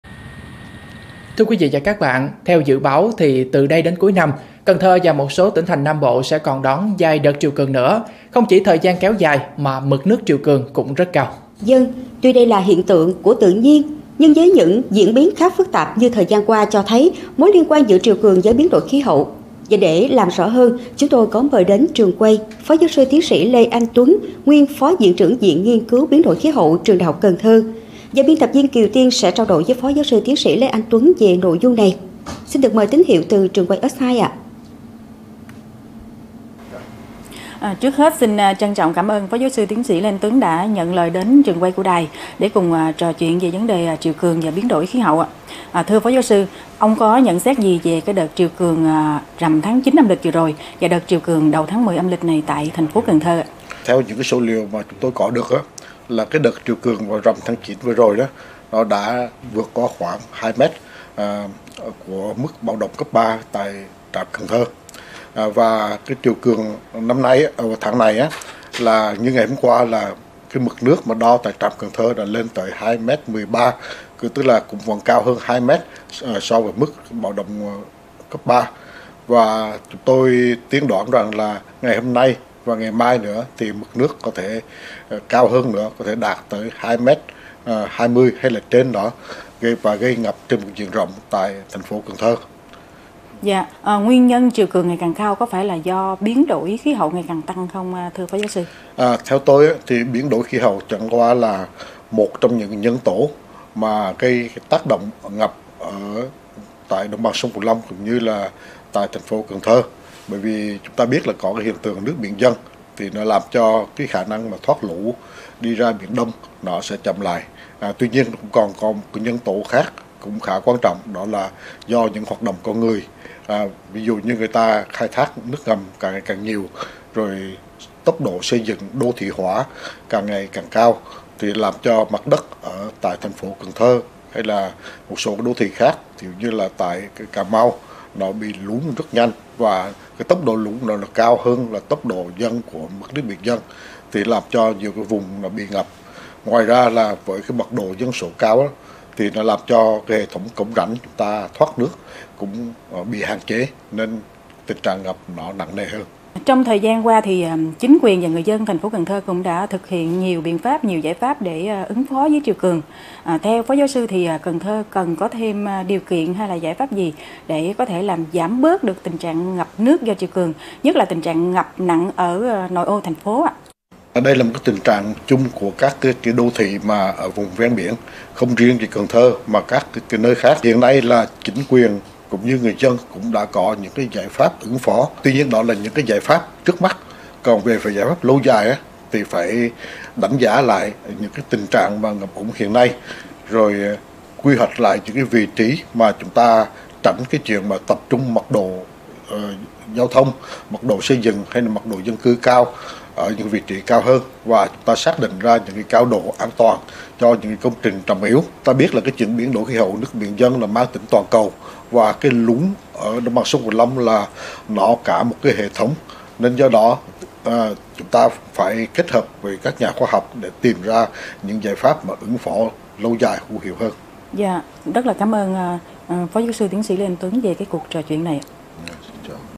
Trò chuyện cùng chuyên gia về vấn đề triều cường-biến đổi khí hậu | Cần Thơ TV
Theo dự báo, từ đây đến cuối năm, Cần Thơ và 1 số tỉnh thành Nam Bộ sẽ còn đón vài đợt triều cường nữa, không chỉ thời gian kéo dài, mà mực nước triều cũng rất cao. Tuy đây là hiện tượng của tự nhiên, nhưng với những diễn biến khá phức tạp như thời gian qua cho thấy mối liên quan giữa triều cường với biến đổi khí hậu.